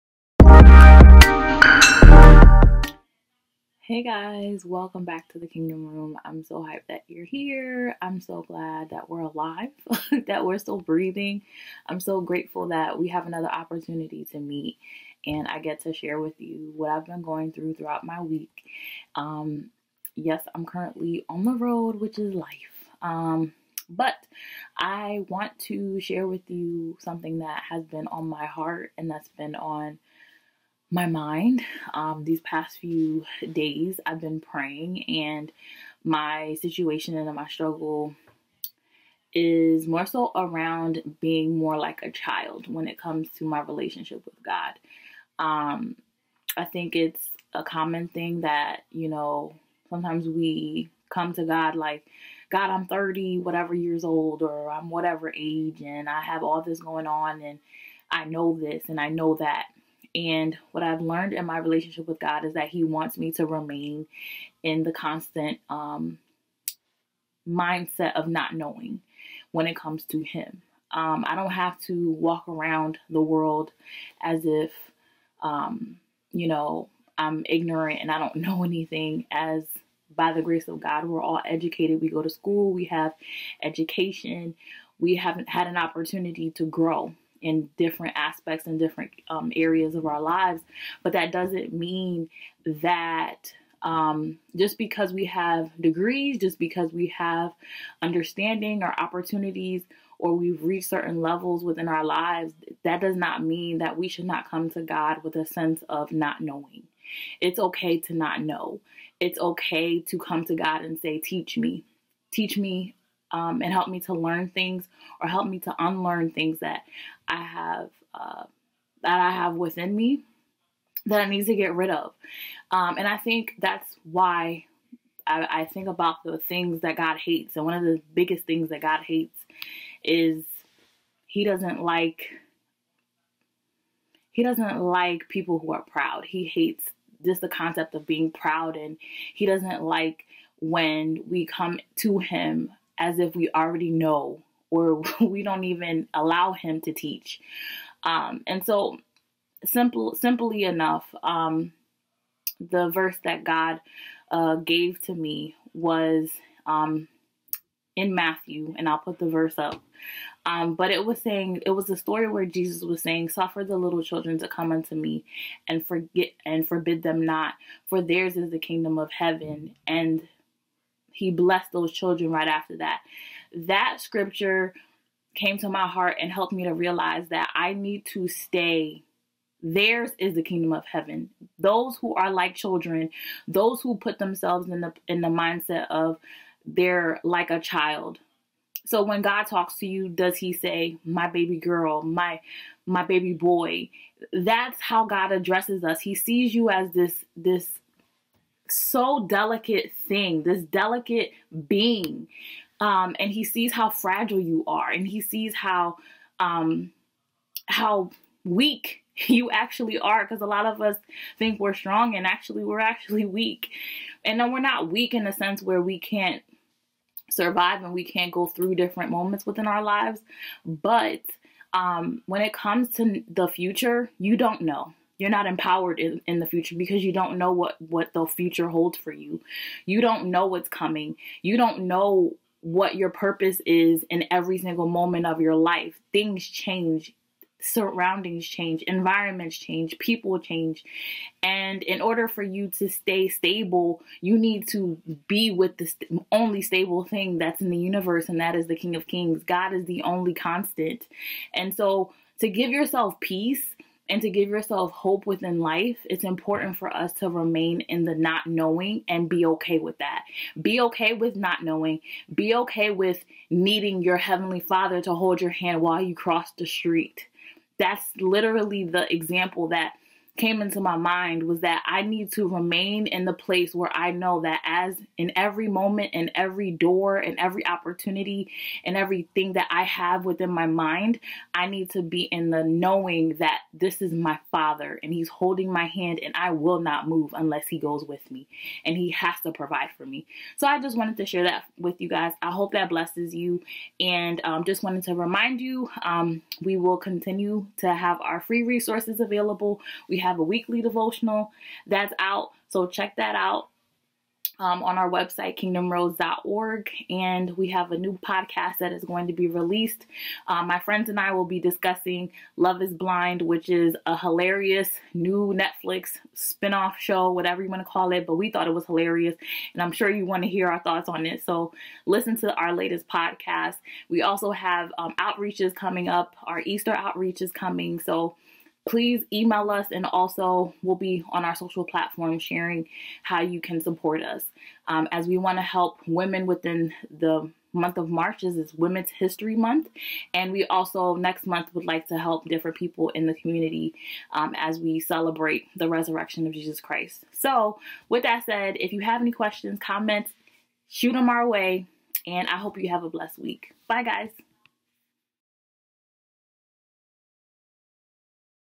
The Struggle of Not Knowing
Mia shares how to be okay with Not Knowing everything...

Visit Kingdomroads.org for more information on our organization.